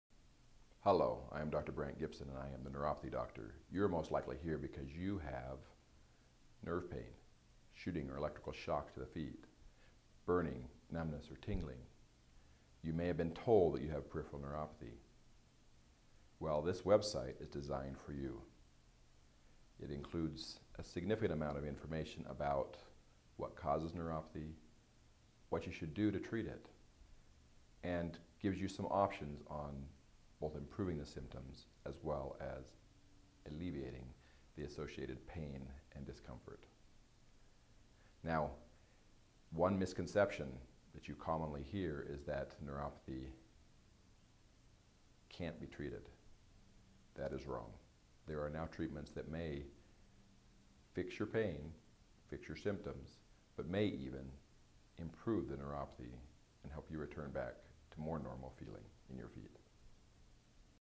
New Options At Peripheral Neuropathy Center of Utah
The Peripheral Neuropathy Center of Utah with Dr Brandt R Gibson, the Neuropathy Doctor is seeking to provide new treatment options and possible solutions to symptomatic peripheral neuropathy.  Are there options for treating your neuropathy?